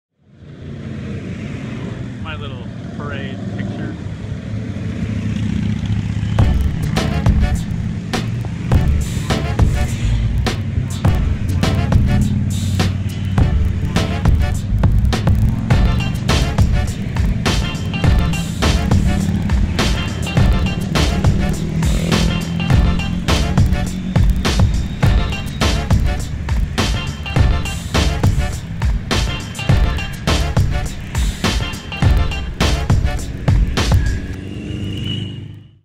Motorcycles Disembarking
Motorcycles get priority boarding on Washington State Ferries. This incentivizes residents who use ferries to commute to work to ride a bicycle or a motorcycle. You can see how many do just that, at least on sunny days.